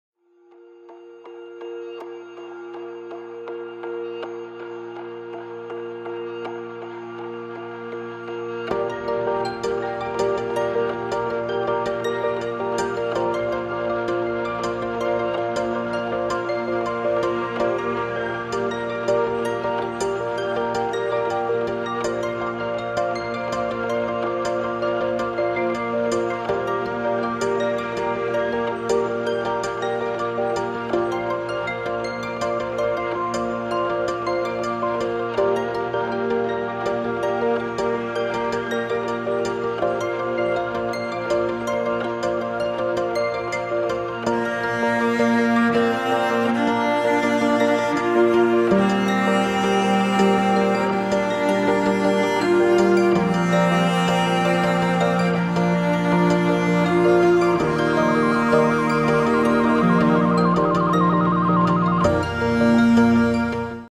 Казахстанцев снова пугают самолетами с химикатами
Сегодня на NurTV News:
Казахстанцы вновь распространяют в мессенджерах "доказательства" химтрейлов. На этот раз свидетельством "распыления ядов" служит видео взлетающего самолета, якобы снятое в Северно-Казахстанской области

А также заходите на наш сайт – самый большой новостной ресурс Казахстана.